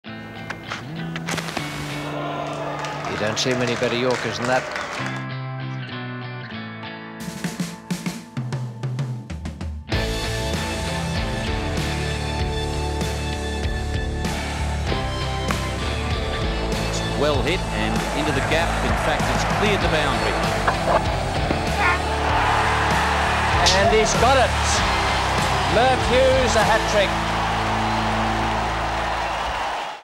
Merv Hughes inducted to Hall of Fame | Australian Cricket Awards 2021
Merv Hughes becomes the 51st member of Australian cricket's Hall of Fame for a 53-Test career that yielded 212 wickets at 28.40, plus 38 wickets in 33 ODIs, and a larger than life personality that makes him an icon of the game.